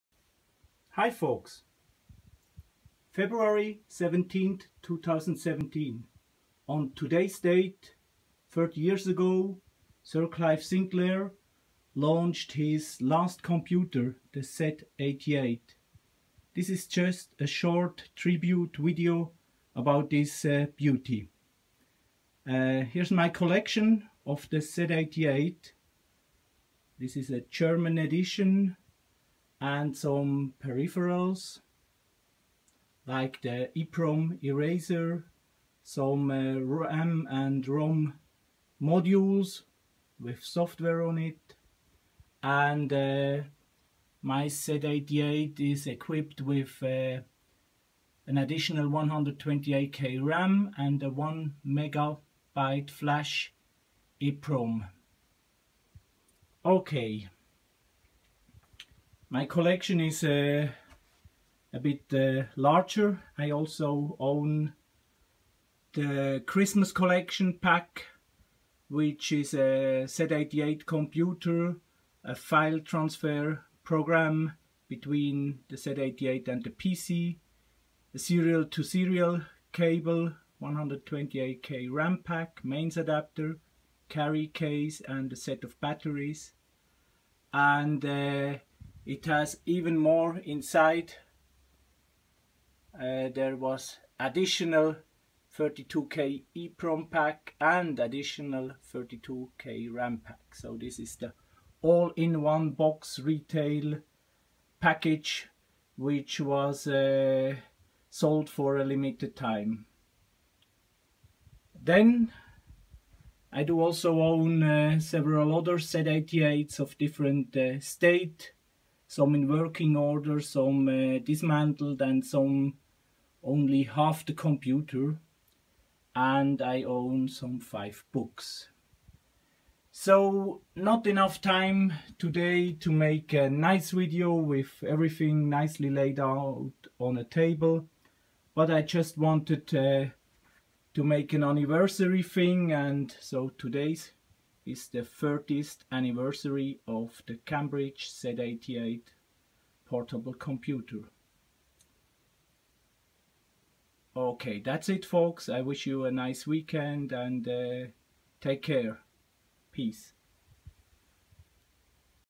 Z88 is 30
30th launch anniversary of the Cambridge Z88 portable computer on February 17th 2017.

ID: MOV135